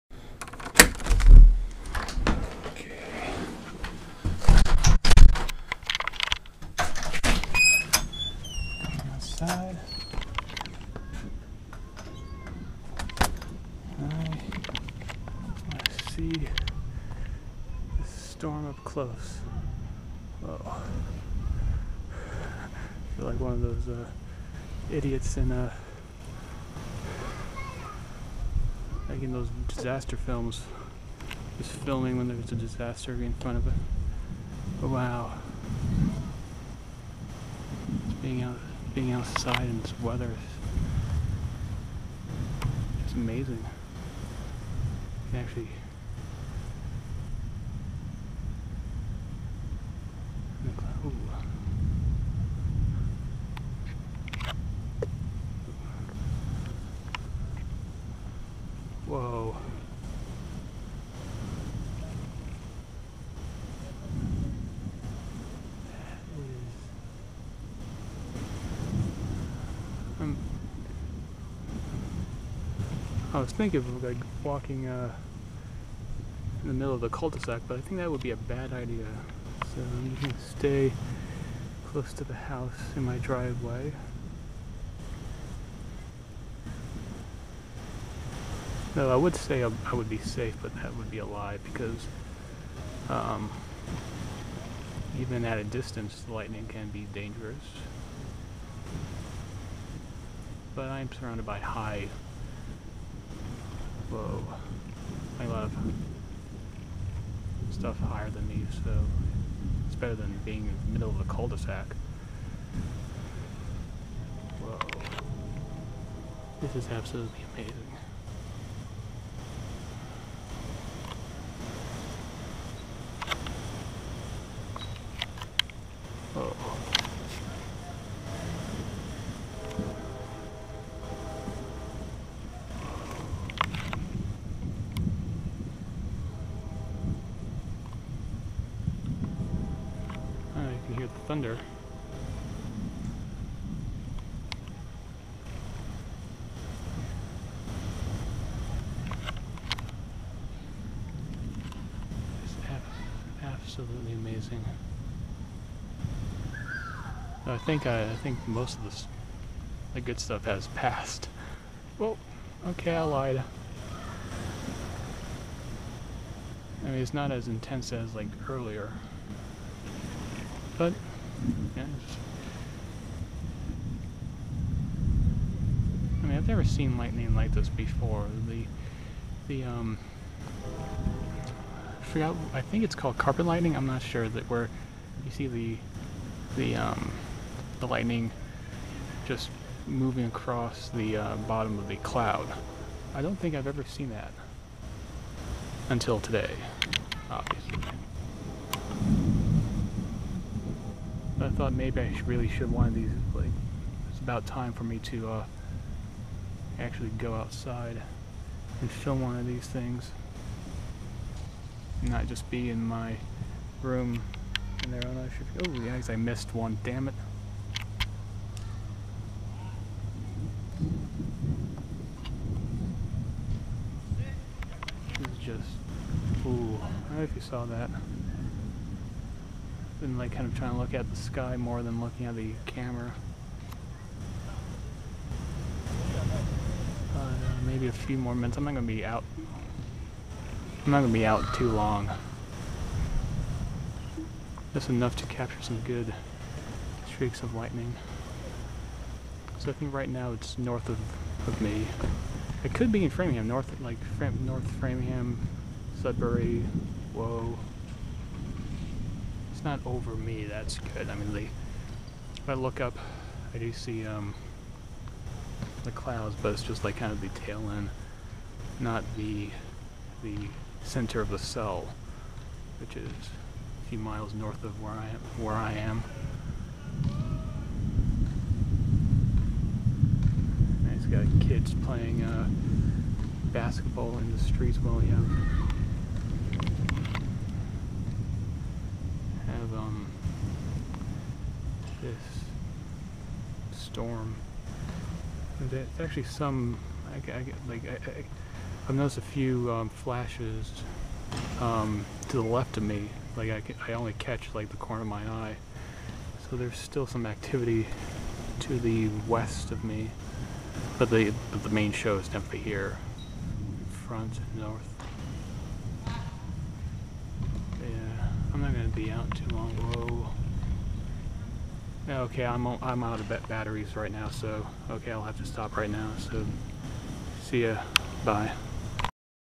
Wacky Weather X: Supercell Part 3 - July 19, 2010 Storm
Part 3 of the powerful thunderstorm I filmed back in July.